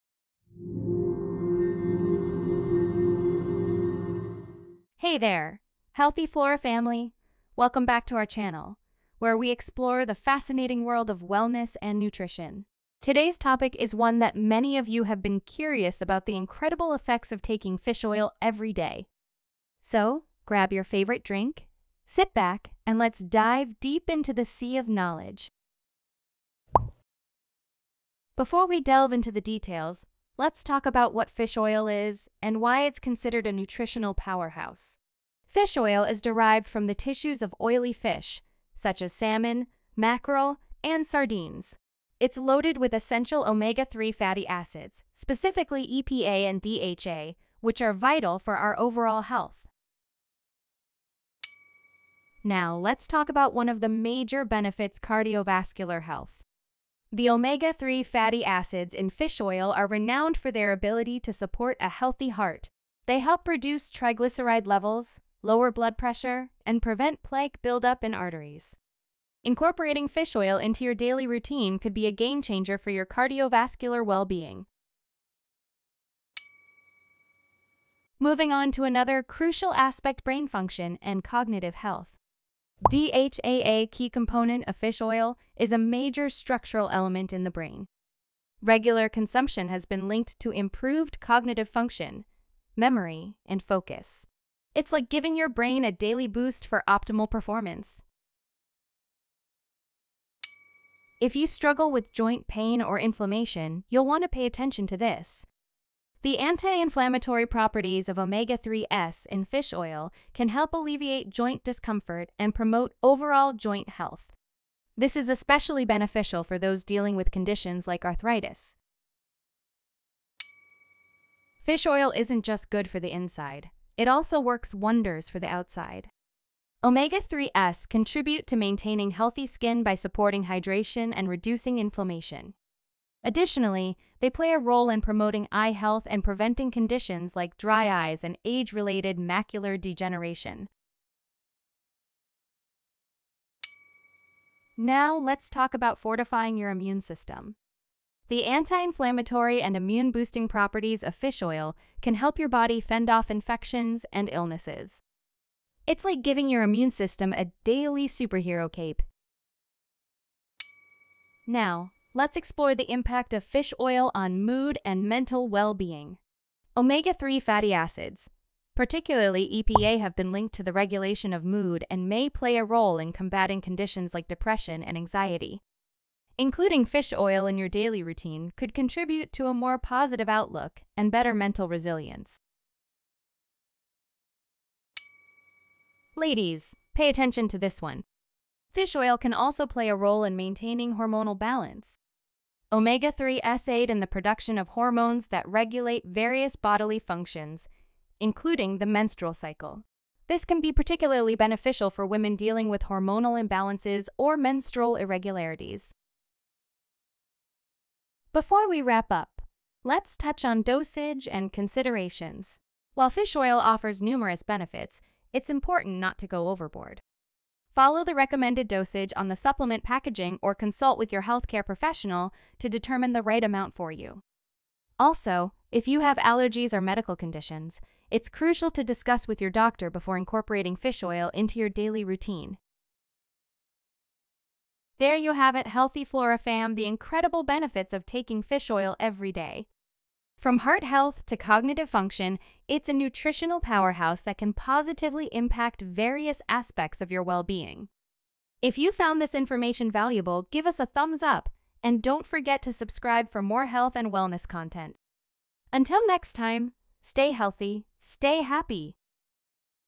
What Happens When You Take Fish Oil Everyday | Omega 3 Fatty Acid
Welcome to Healthy Flora, your go-to destination for insightful information on health and wellness! In today's video, we'll delve into the fascinating topic of what happens when you make fish oil a daily part of your routine. Fish oil is renowned for its potential health benefits, and incorporating it into your daily regimen can lead to a myriad of positive effects.

Key Points Covered in This Video:

1. Cardiovascular Health: Discover how the omega-3 fatty acids found in fish oil can contribute to a healthy heart and support cardiovascular well-being.

2. Cognitive Function: Uncover the potential cognitive benefits of regular fish oil consumption, including improved memory and cognitive function.

3. Joint Health: Explore how fish oil may aid in reducing inflammation and promoting joint health, benefiting individuals dealing with joint discomfort.

4. Skin and Hair: Learn about the potential impact of fish oil on skin health, including its moisturizing properties and potential benefits for hair health.

5. Mood and Mental Health: Delve into the relationship between fish oil and mood regulation, examining how it may positively influence mental health.

6. Immune System Support: Understand how fish oil may contribute to a robust immune system, helping your body defend against various illnesses.

7. Dosage and Considerations: Gain insights into recommended dosage, potential side effects, and considerations when incorporating fish oil into your daily routine.

Remember, while fish oil offers numerous health benefits, it's crucial to consult with a healthcare professional before making significant changes to your supplement regimen.

Empower yourself with knowledge, and let's embark on a journey to a healthier, happier you!